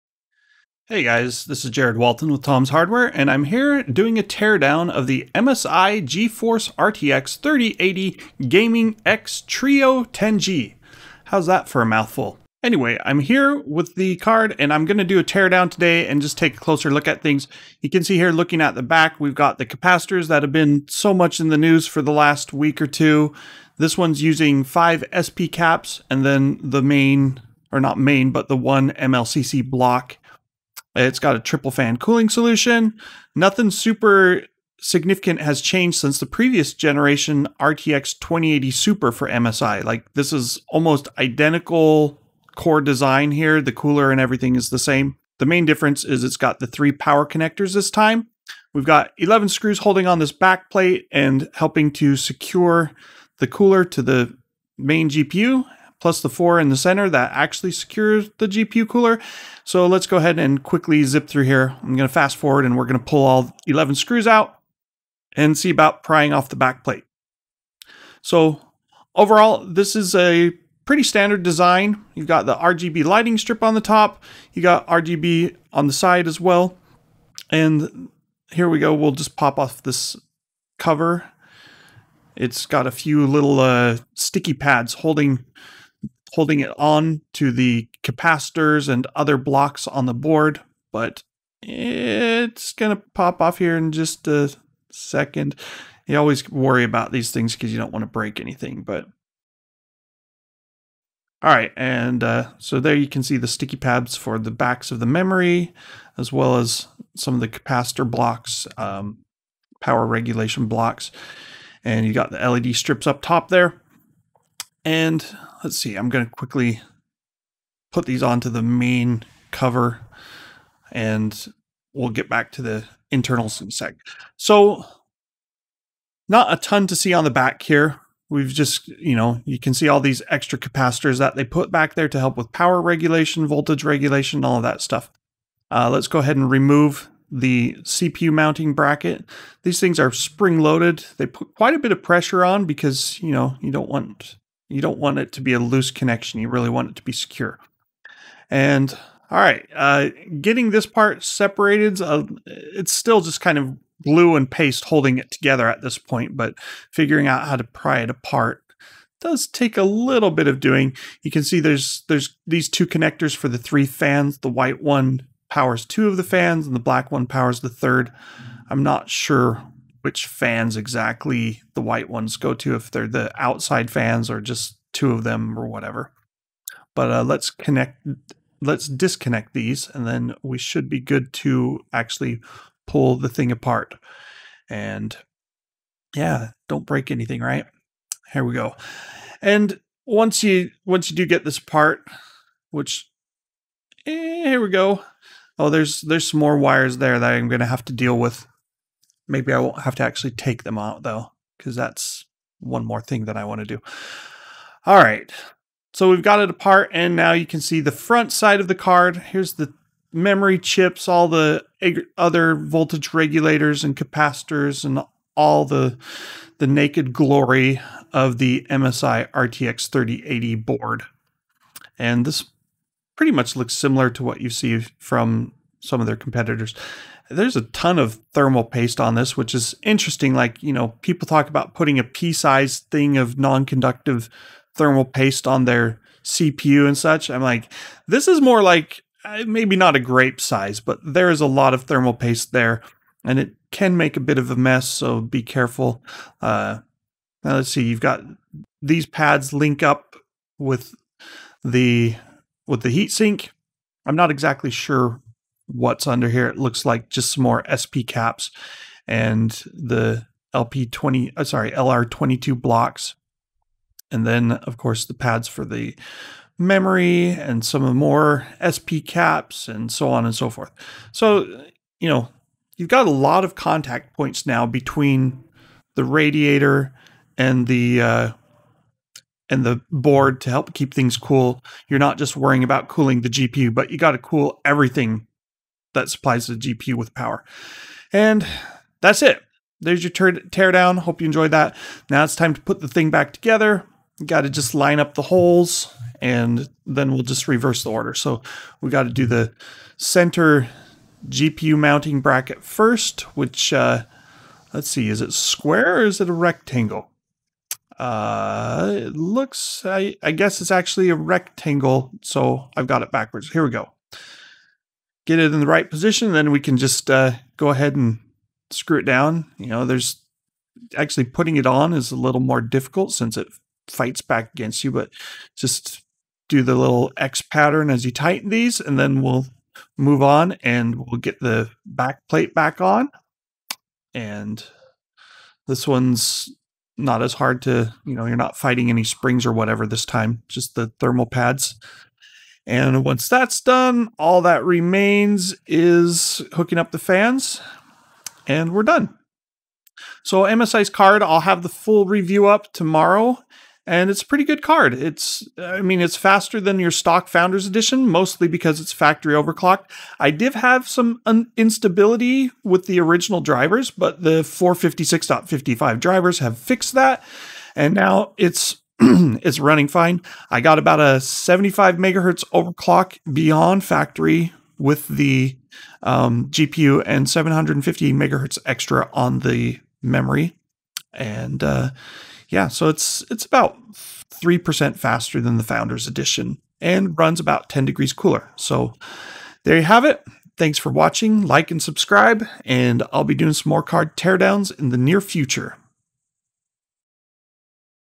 MSI GeForce RTX 3080 Gaming X Trio Teardown
We take a quick look at the PCB and other components hiding under the heatsink and backplate on the MSI RTX 3080 Gaming X Trio. This is a large custom GPU, with ample cooling and triple 8-pin power connectors